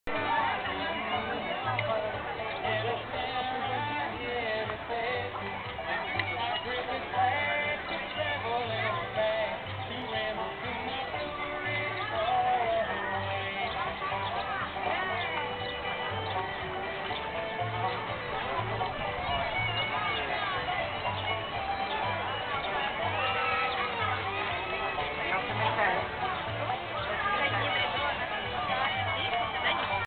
The Best Ride in PortaAventura Universal Spain. The Hurrican
Best of the Big 4. The Hurakan Condor. 100metres high freefall. (With tipping seats-if your lucky